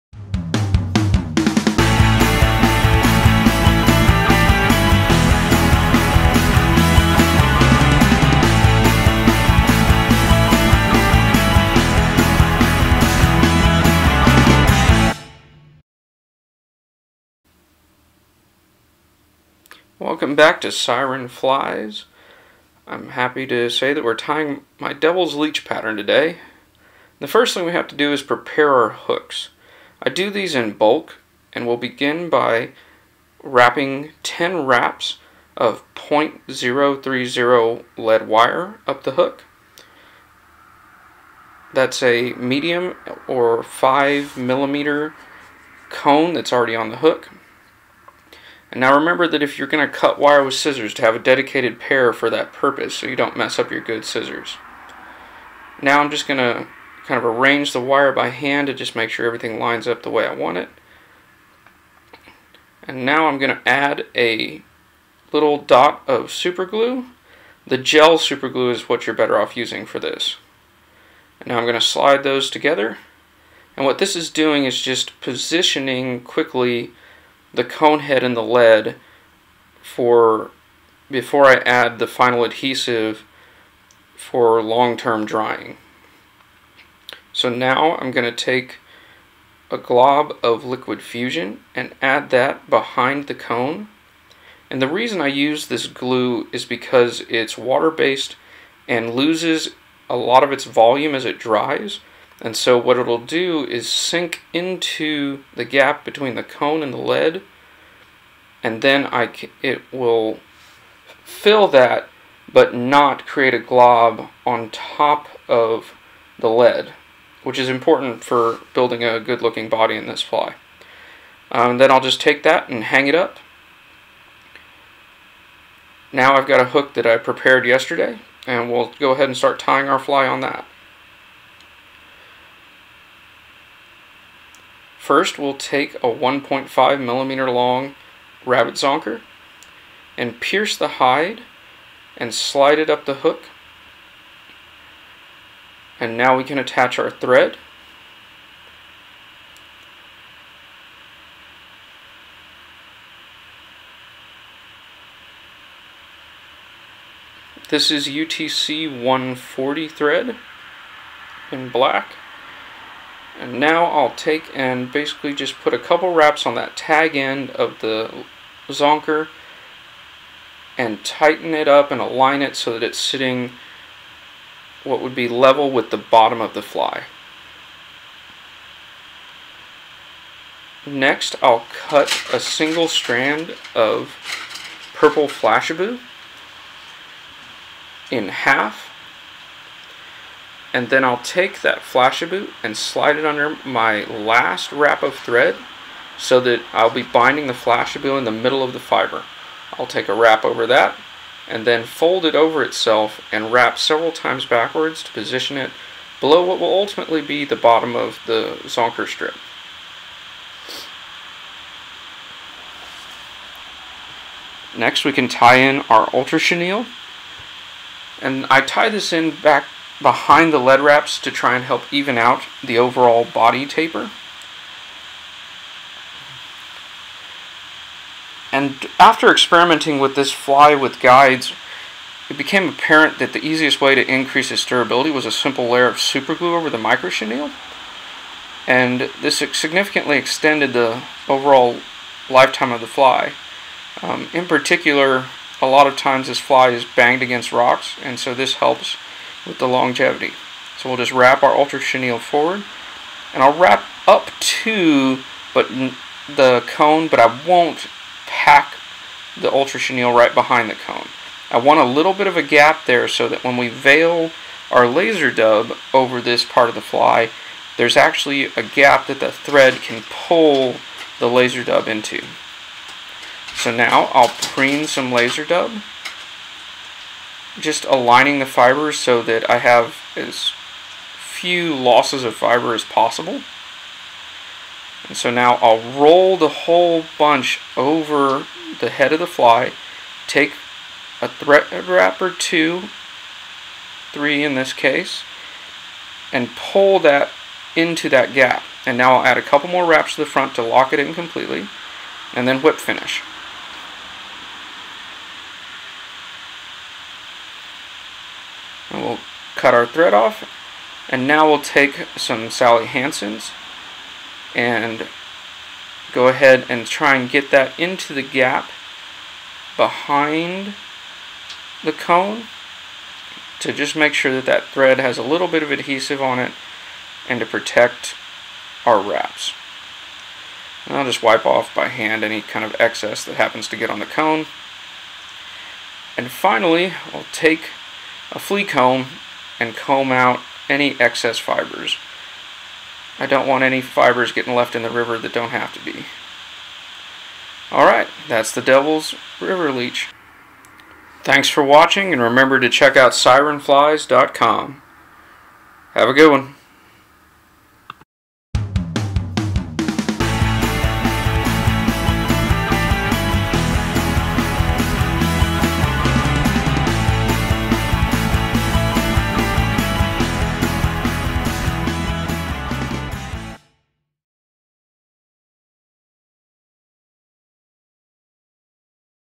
Devils Leech
The devils leech is an all around patter that will catch most anything. It was designed in collaboration with guide Daniel Hughes to be a fly that would take on any fish in the Devils River without needing to change flies.